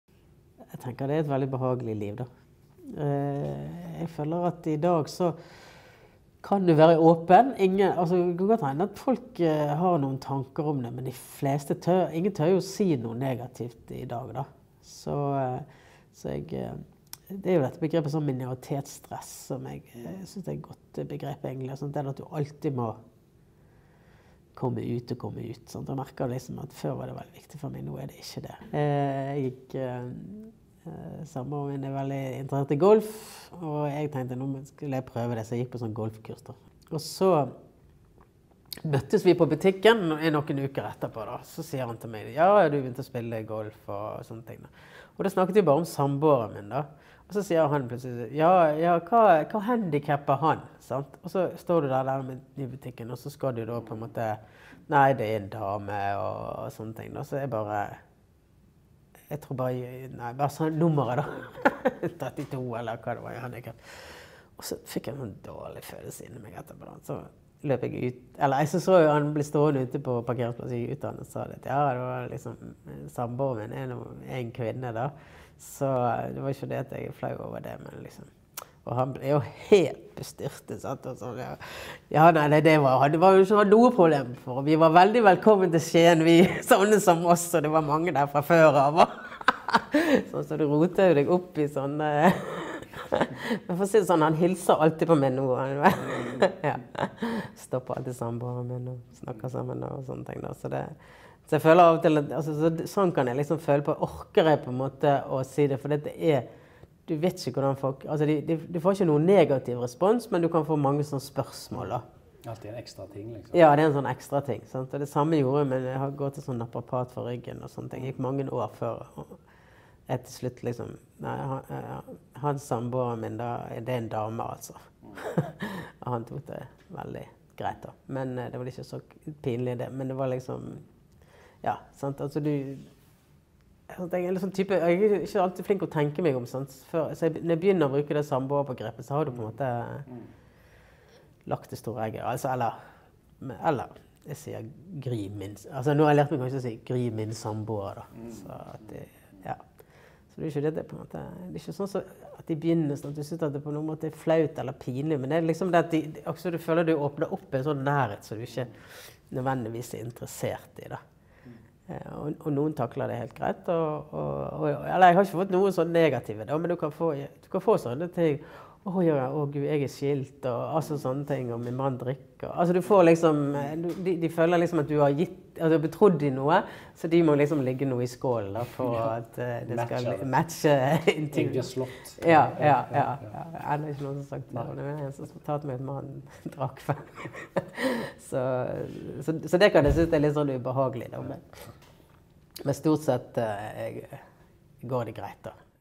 - Minoritets stress.
- "Minoritets-stresset". Du må komme ut hele tiden.

Kjersti Ulriksen var aktiv i DNF-48 i Bergen på 80-tallet.

I dag bor hun i Skien med sin samboer. Her forteller hun om hvordan det er å komme ut i alle sammenhenger.